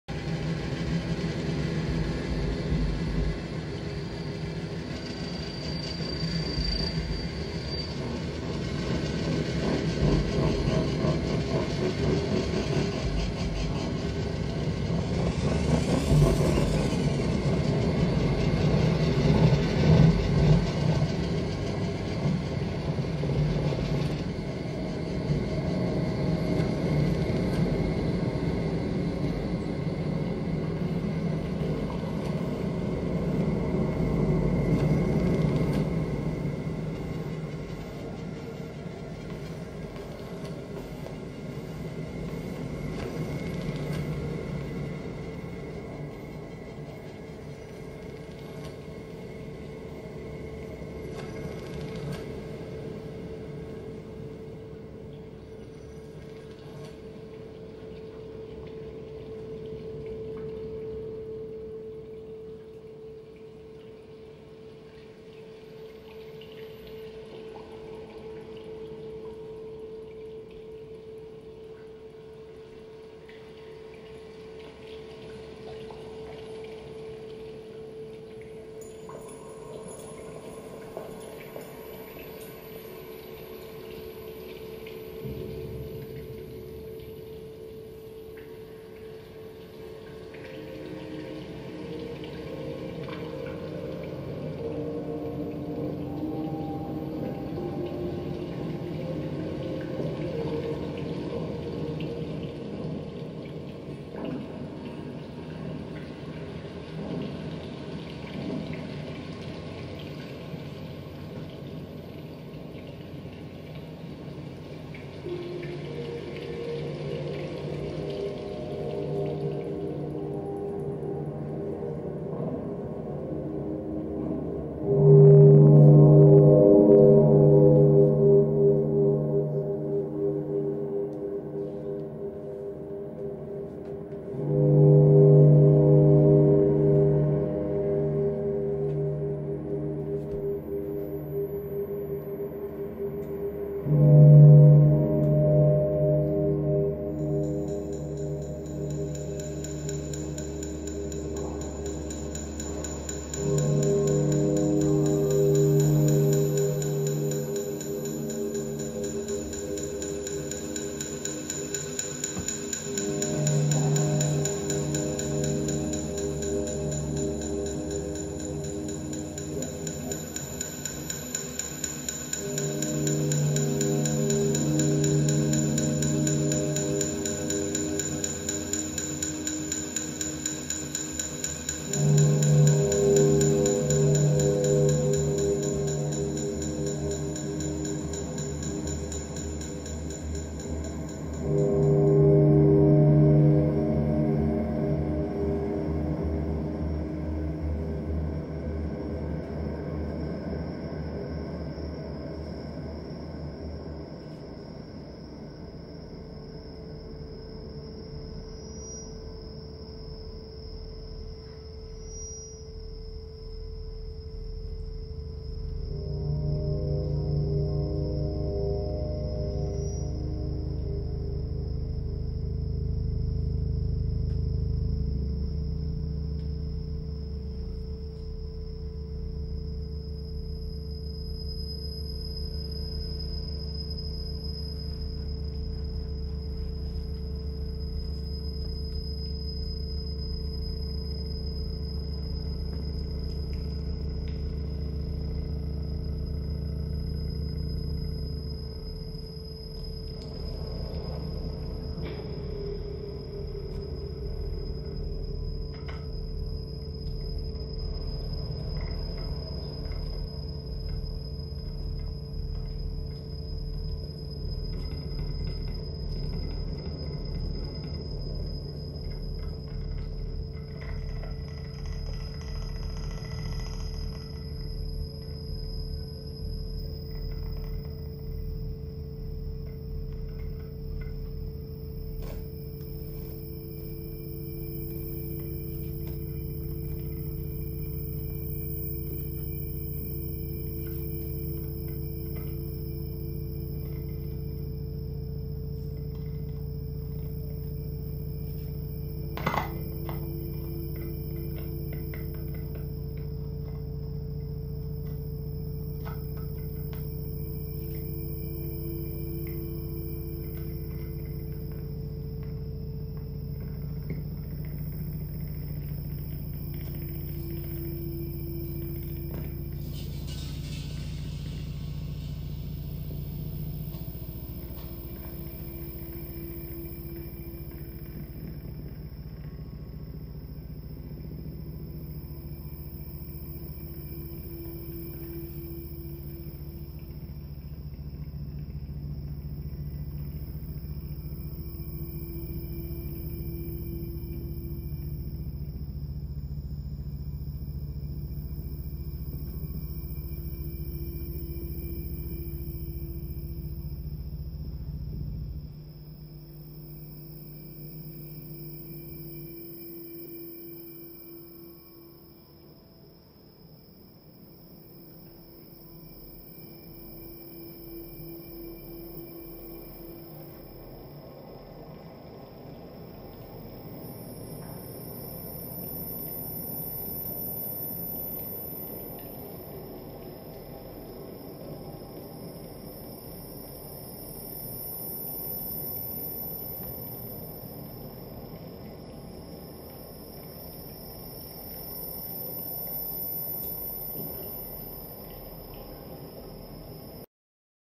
Christophe Charles Quintet at Mopomoso
Christophe Charles (born Marseille 1964), works with found sounds, and makes compositions using computer programs, insisting on the autonomy of each sound and the absence of hierarchical structure. Here he plays at Mopomoso the free improvisation night at the Vortex jazz club in London with yasutaka Watanabe and Hoonoda Kim, also on laptops and electronics, with dancers Lisa Koiso and Woo Kyung Son.